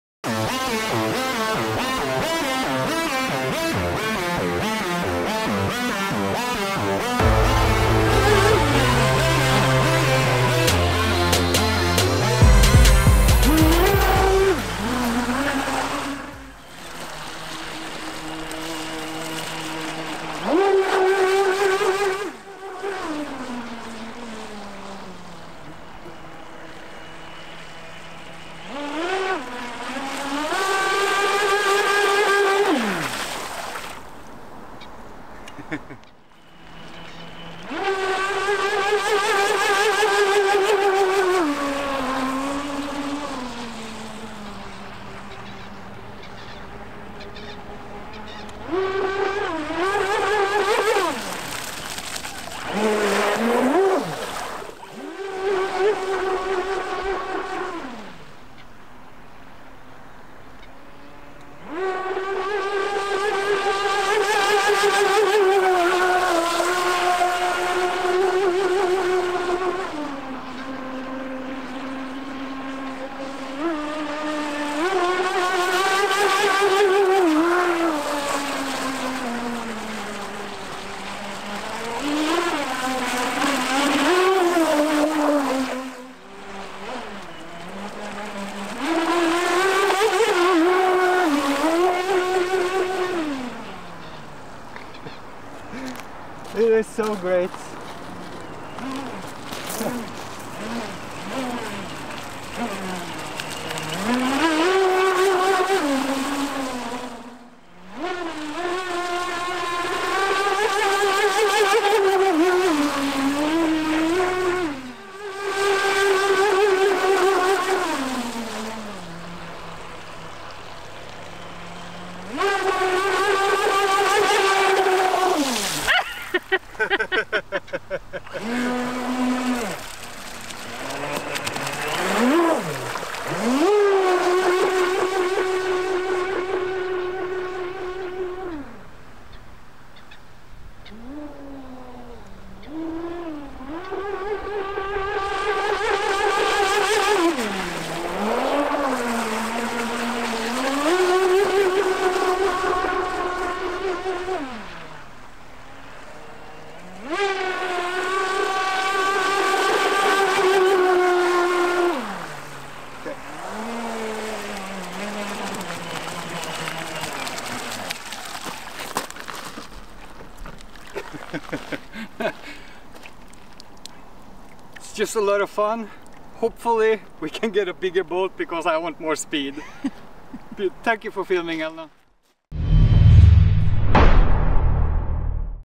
Joysway Monster Cat - Testing speed and new camera. UHD 4K
Elna just got a new camera and are still trying out how to use it. She is doing a great job but still a bit shake here and there but she knows what she is doing wrong and we talk alot about the handling so i know she will nail it in the end.
The Monster cat is so much fun but a bit slow for my taste. Hoping to get my self a bigger one soon. This video is without slow motion just speed passes and good fun.

DJI Mini 3 Pro incl. DJI RC

Camera: Panasonix HC-X2000
Behind camera: Elna
Edit: Bengt
Pilot: Bengt